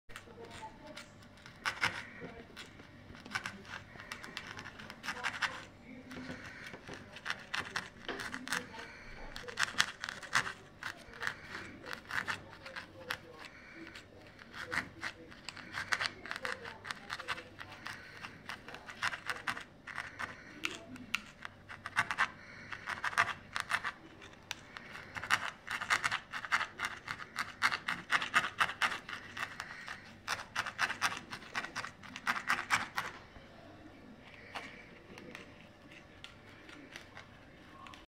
Bromtrol naaldvilten
Bekijk hier de werkwijze de Bromtrol maken d.m.v. Naald Vilten